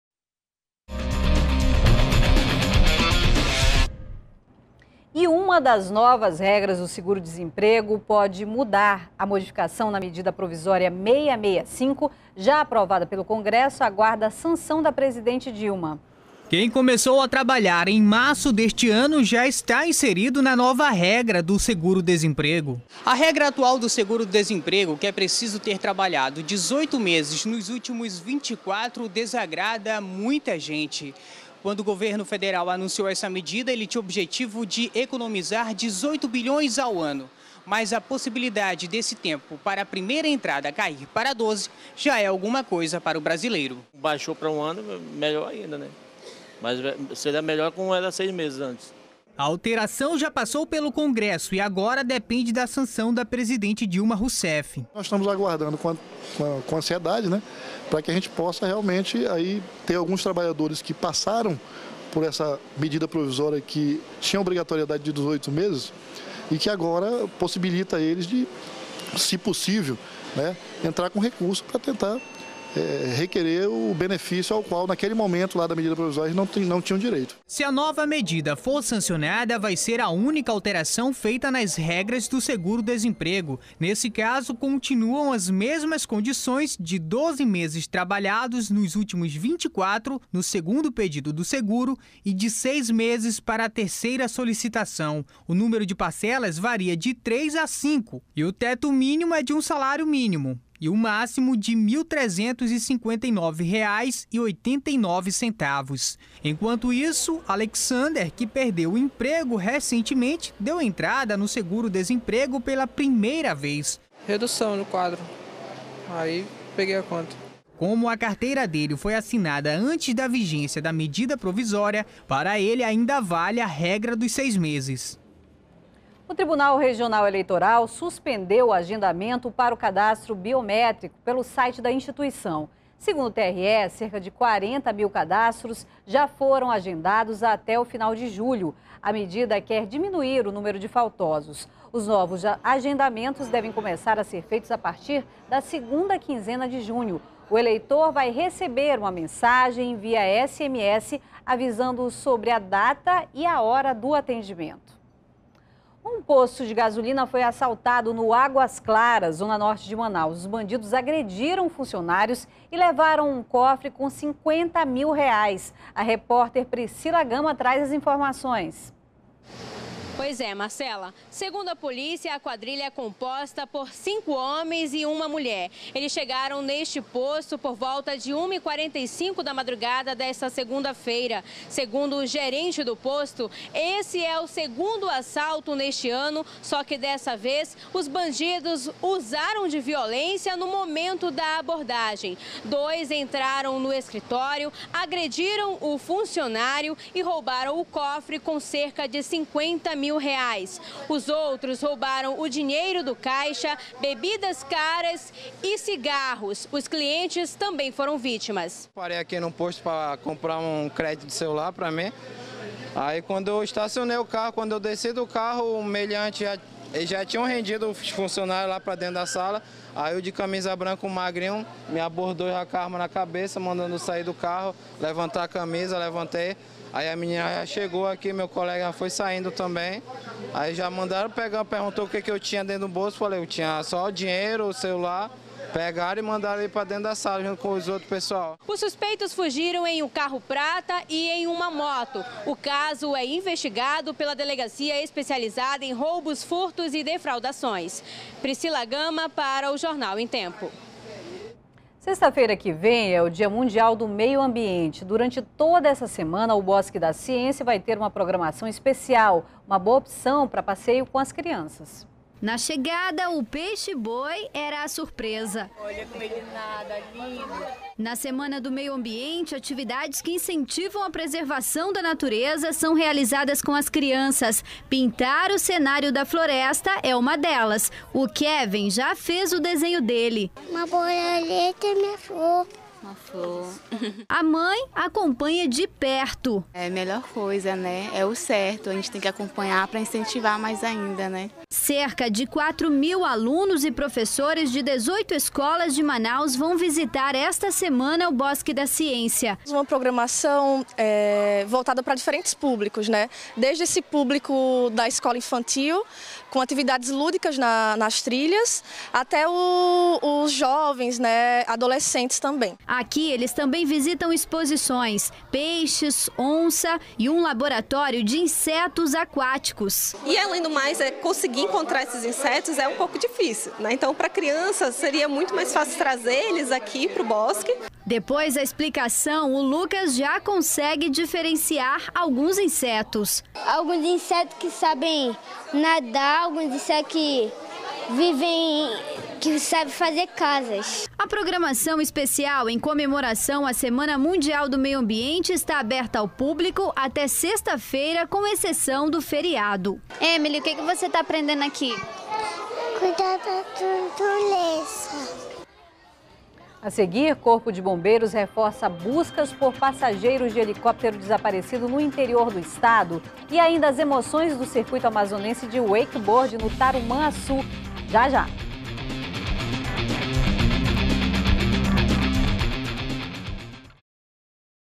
Bandidos levam cofre com R$ 50 mil de posto de gasolina no Águas Claras
02º BLOCO JORNAL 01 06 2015 -  Bandidos levam cofre com R$ 50 mil de posto de gasolina no Águas Claras